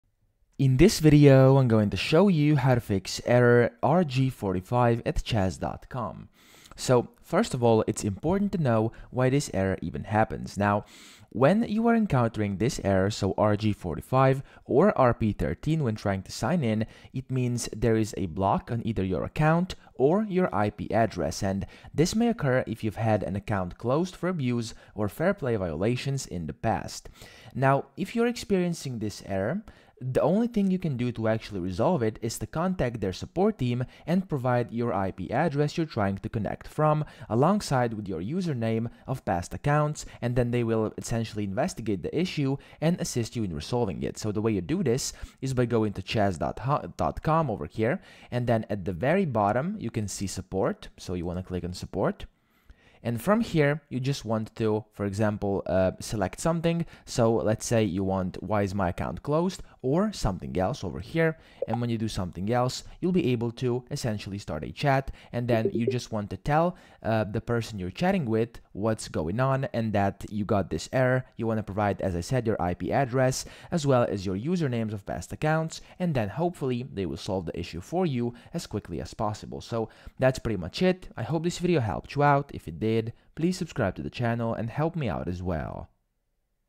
How To FIX Error RG45 At Chess.com (WORKING) 2026
In this video, I'll show you how to fix the Error RG45 on Chess.com. This is a full guide on how to resolve this error and get back to playing in 2026.

If you've been struggling with this issue on Chess.com, this is the video you need to see!